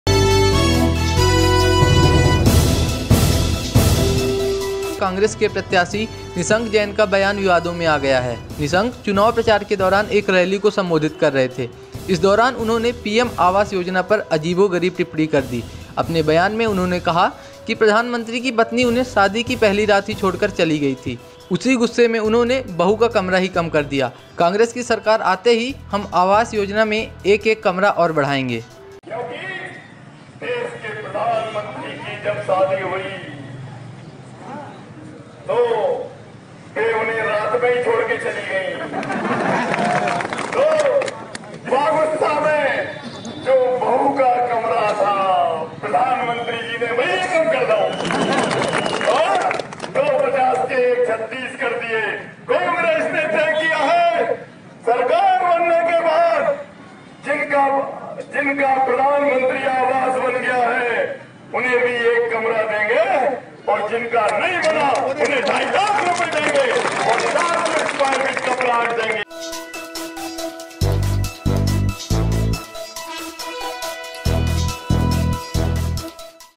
ये क्या कह गए निशंक जैन
कांग्रेस के प्रत्याशी निशंक जैन का बयान विवादों में आ गया है। निशंक चुनाव प्रचार के दौरान एक रैली को संबोधित कर रहे थे। इस दौरान उन्होंने  पीएम आवास योजना पर अजीबोगरीब टिप्पणी कर दी। अपने बयान में उन्होंने कहा की  प्रधानमंत्री की पत्नी उन्हें शादी की पहली रात में ही छोड़ कर चली गईं थी  उसी गुस्सा में उन्होंने बहु का कमरा ही कम कर दिया । कांग्रेस की सरकार आते ही हम आवास योजना मैं एक एक कमरा और बढाएंगे ।